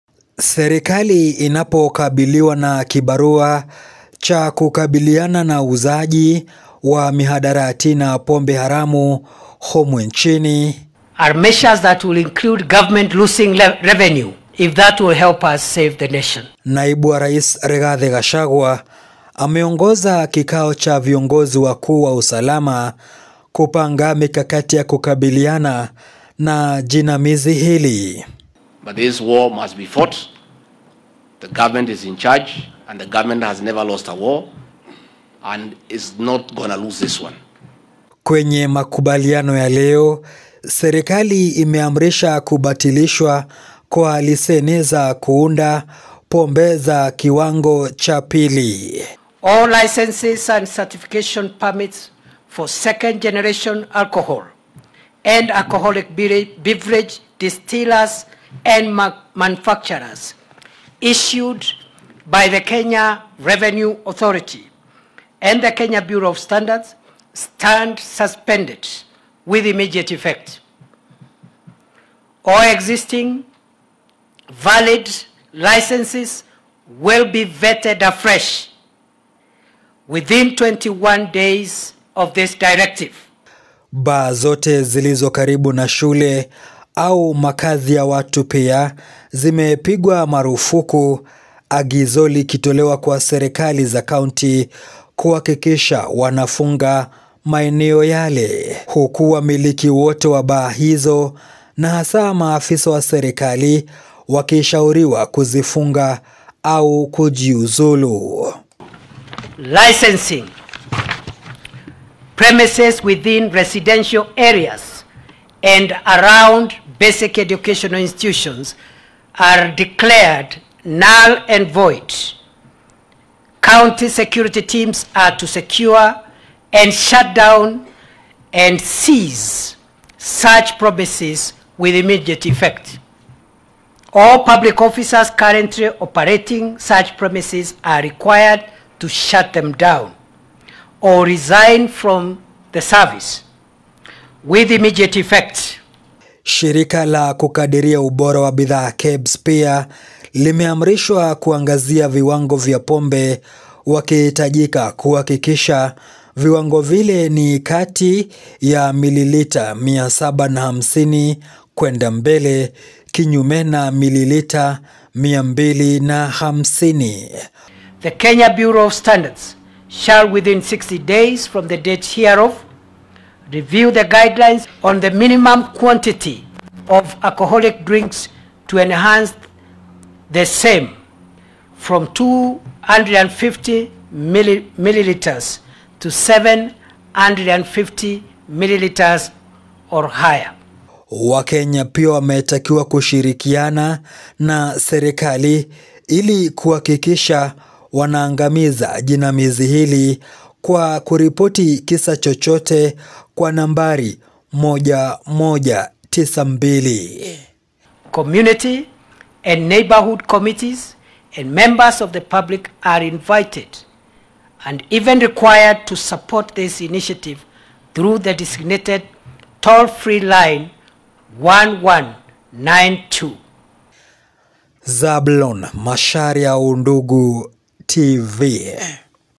MIKAKATI YA KUKABILI POMBE HARAMU
Kindiki: Baa zote zilizokaribu na shule au makazi zifungwe mara moja